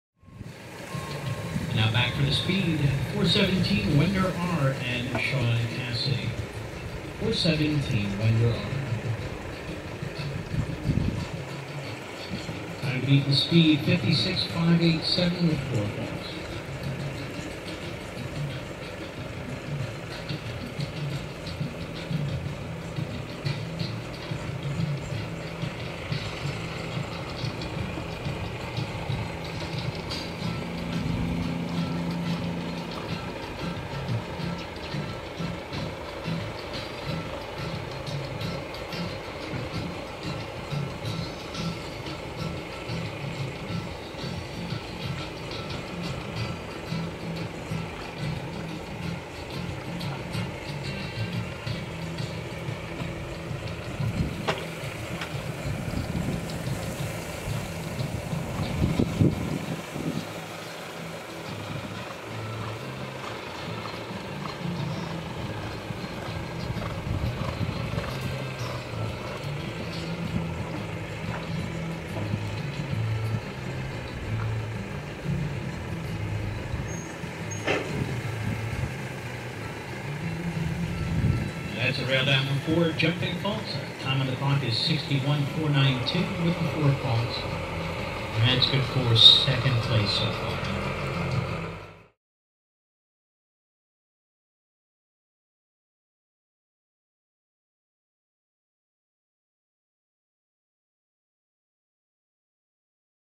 Wender R Ridden by Shawn Casady | 1.30M Jumper Speed Class
Wender R Ridden by Shawn Casady.
The Ridge at Riverview, October 20th 2016.